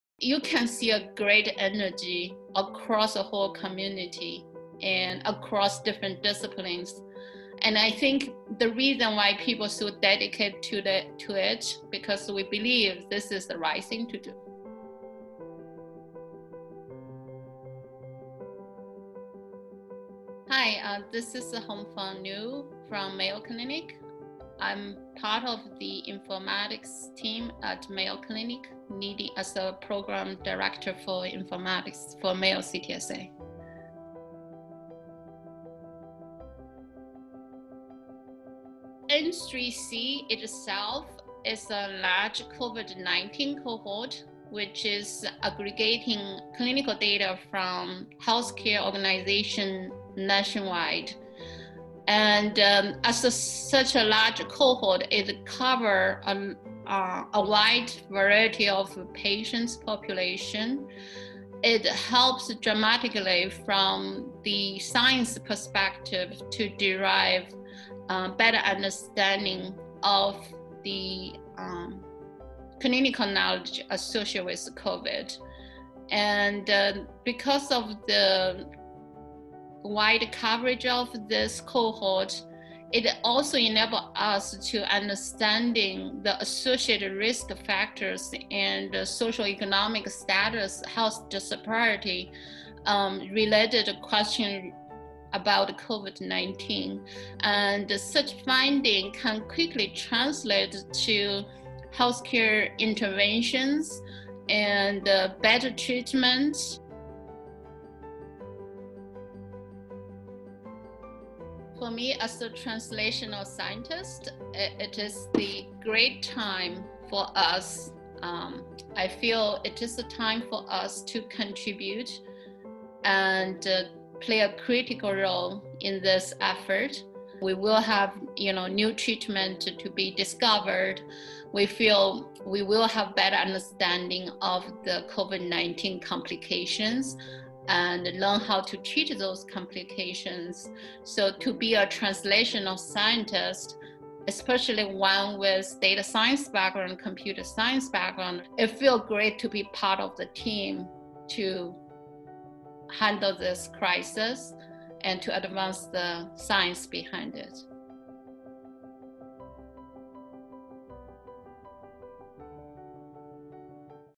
I Am Translational Science: Hongfang Liu, Ph.D., Mayo Clinic
Hongfang Liu, Ph.D., program director for informatics at Mayo Clinic, is experienced in using clinical data for translational science research and improving health care delivery. Dr. Liu explains how collaborating with experts in other disciplines to build the National COVID Cohort Collaborative (N3C) Data Enclave will advance the science behind COVID-19 to deliver health care interventions and treatments.